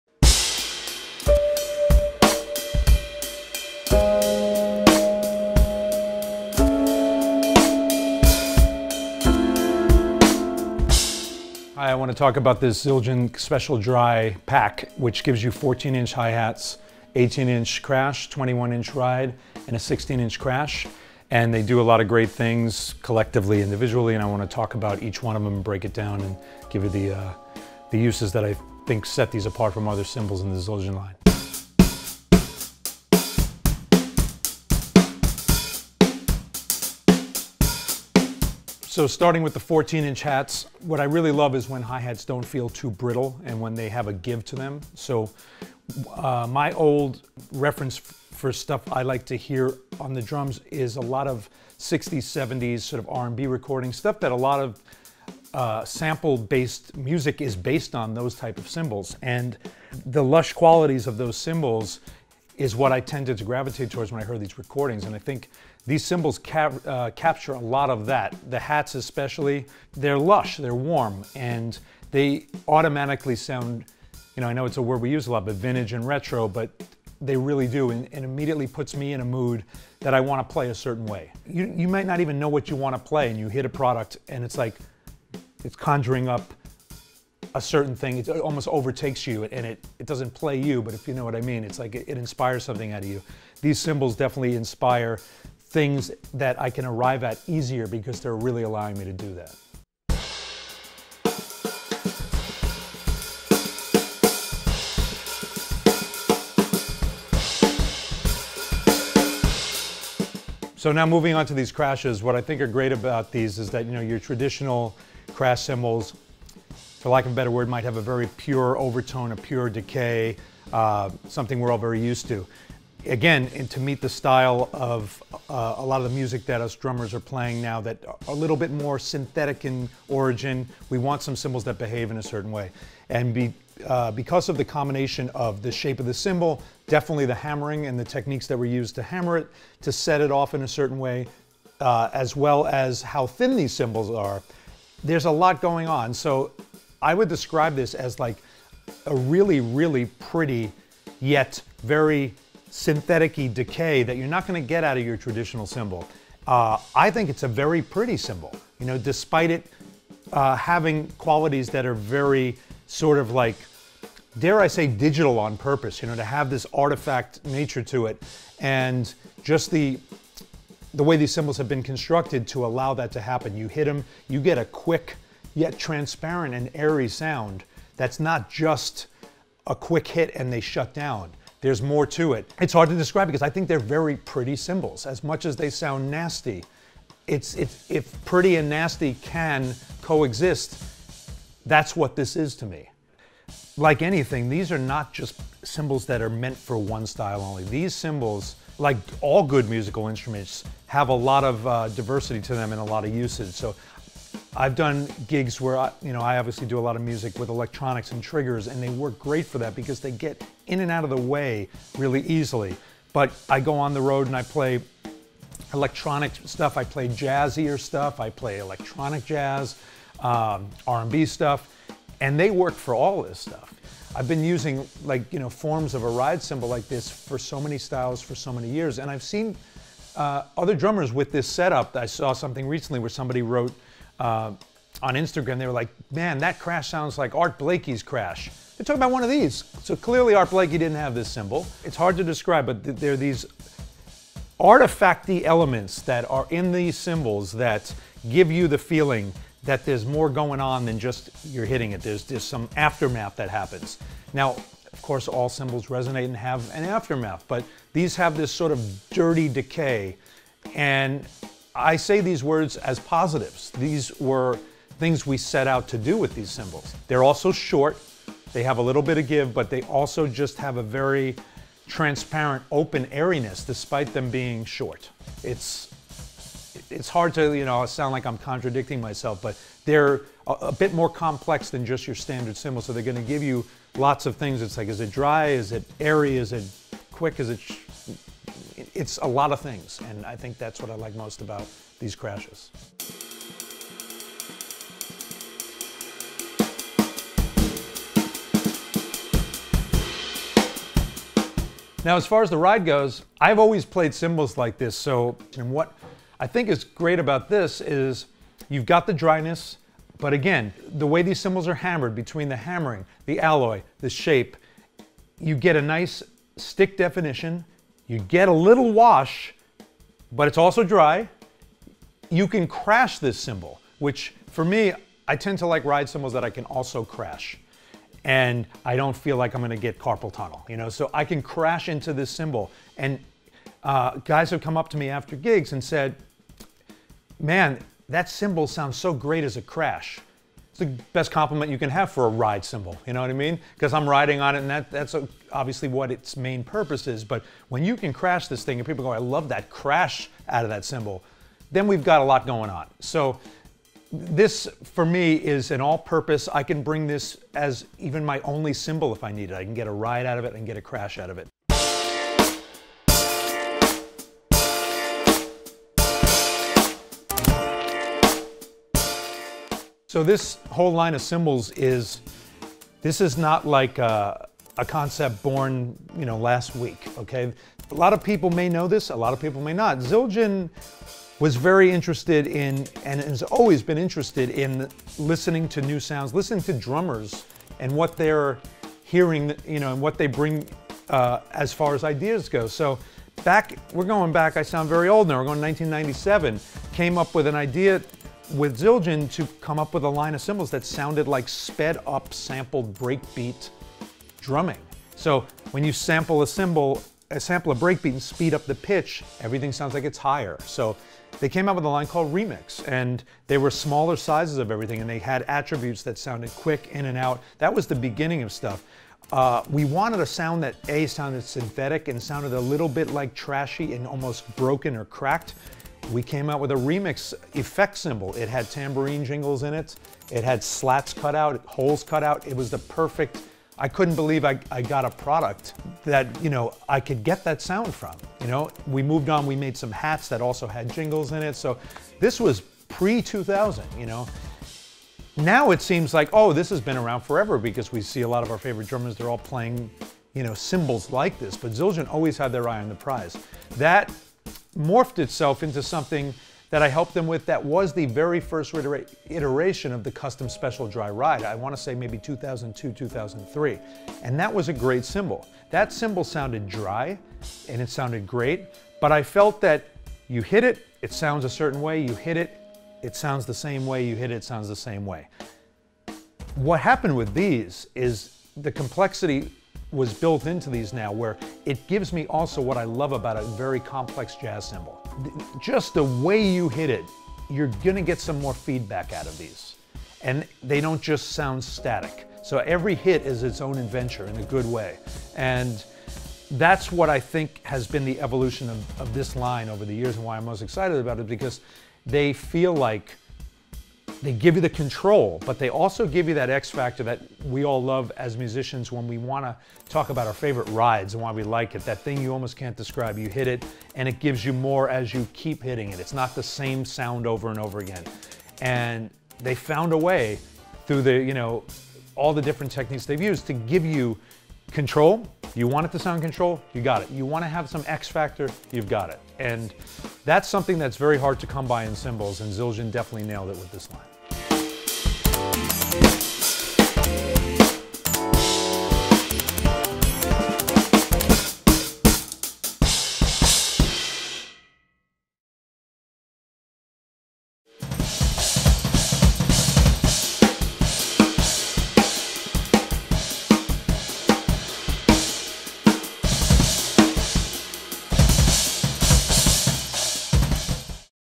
K Custom Special Dry Cymbal Set - Zach Danziger
Zildjian Artist Zach Danziger takes us through the K Custom Special Dry Cymbal Set.

This set is the perfect setup for any drummer who is looking for raw, dry cymbals with quick attack and fast decay.  Dry cymbals are great for the studio, electronic music, hip hop, jazz, funk and more.

THE DRY CYMBAL SET INCLUDES:
14” K Custom Special Dry Hi Hats
16” K Custom Special Dry Crash
18” K Custom Special Dry Crash
21” K Custom Special Dry Ride